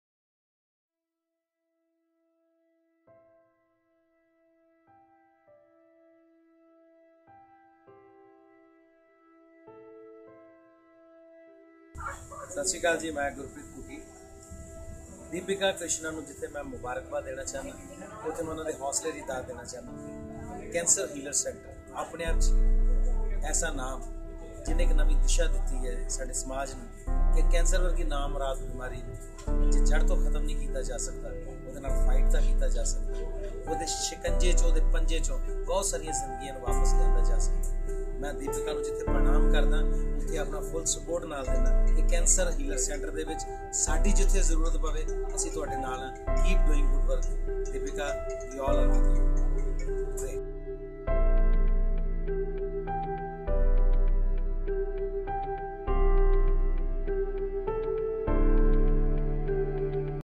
Gurpreet Ghuggi supports Lavender Ribbon Movement
Actor GurpreetGhuggi believes we can BeatCancerTogether and he is with CancerHealerCenter in Lavender Ribbon Movement. What About you?